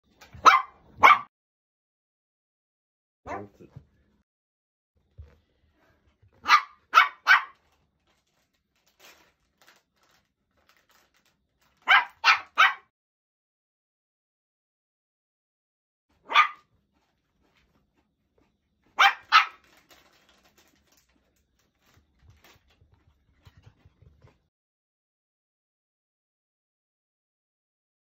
吠えまくるメルセンヌ
何に吠えてるかわかりませんが吠えまくるメルちゃん。
甲高いこてさえだーねー